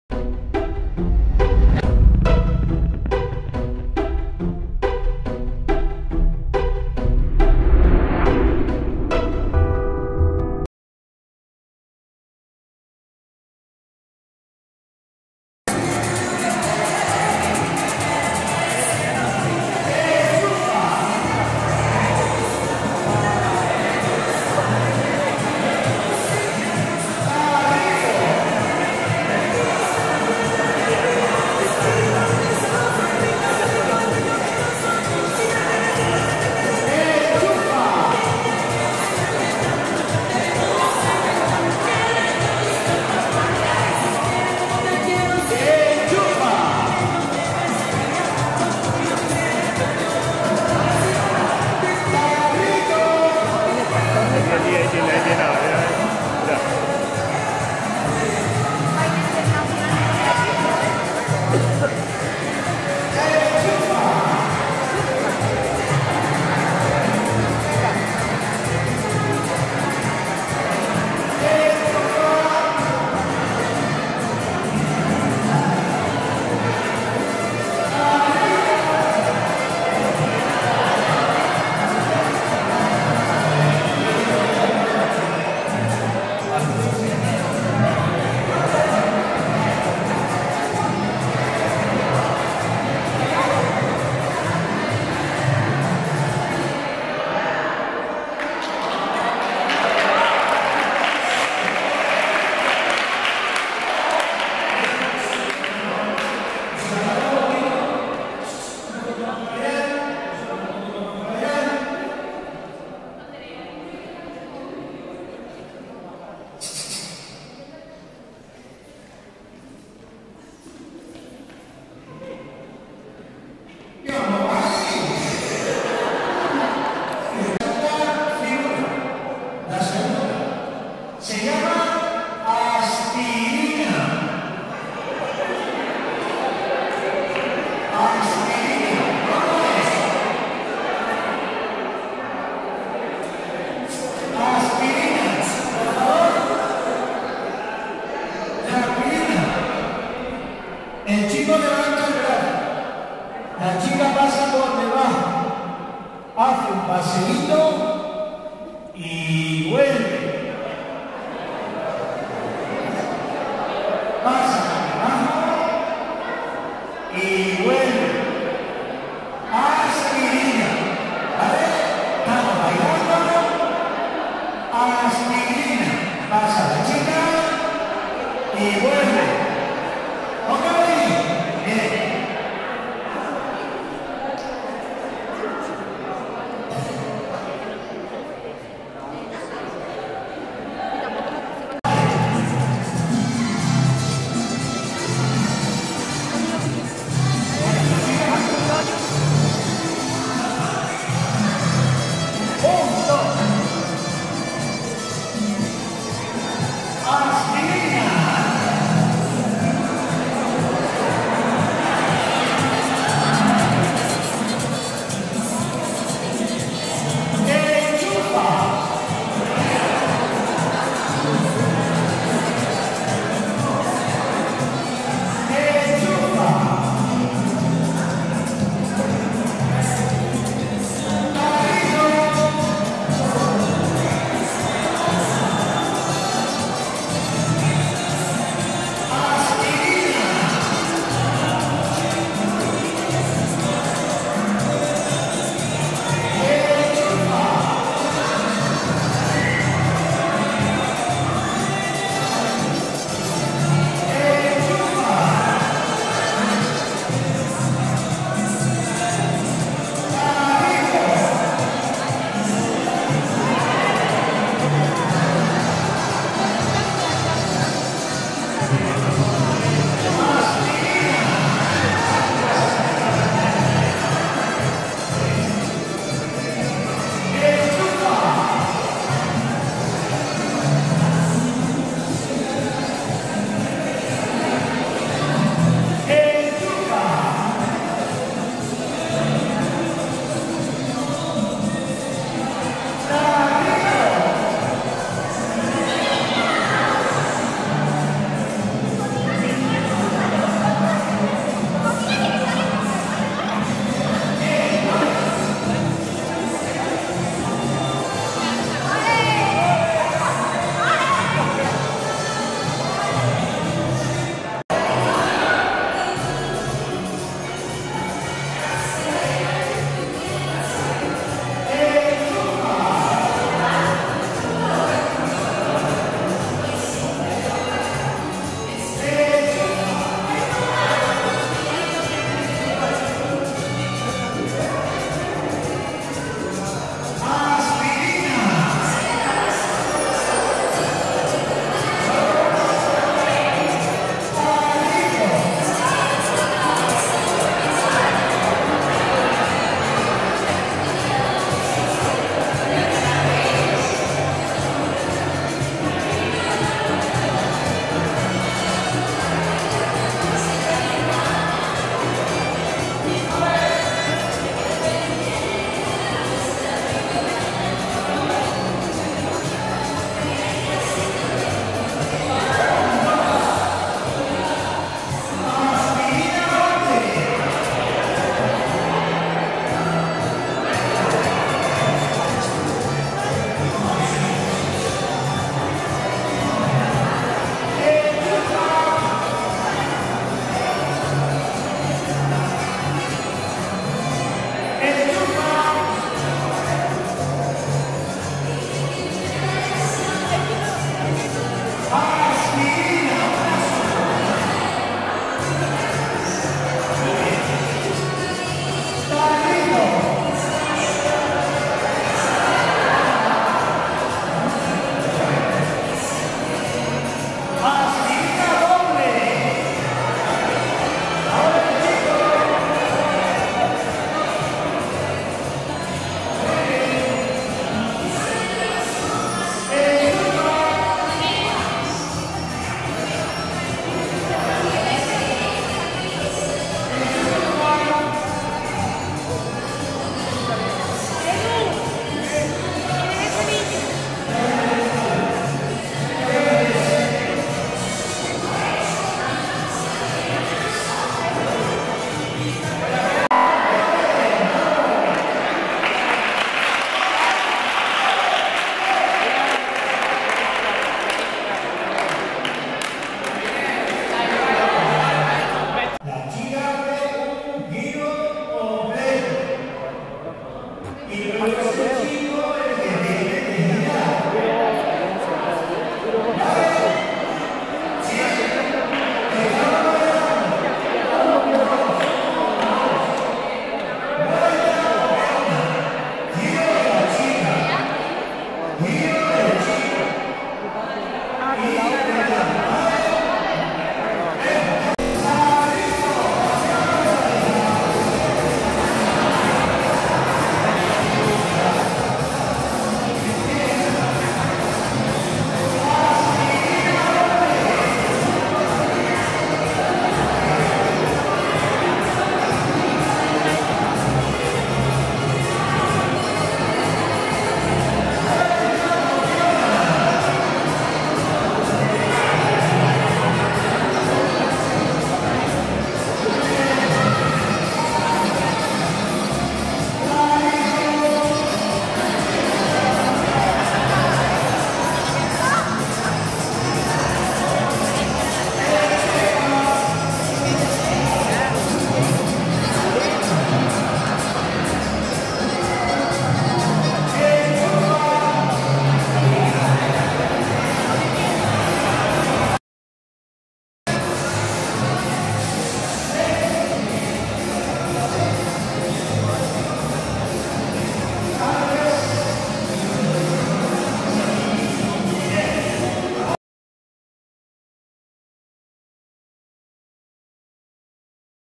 INTERCAMBIOS ITALIA Y EEUU. BAILES LATINOS.
El alumnado de los intercambios de Italia y Estados Unidos, junto al alumnado del Ítaca, aprenden los bailes latinos.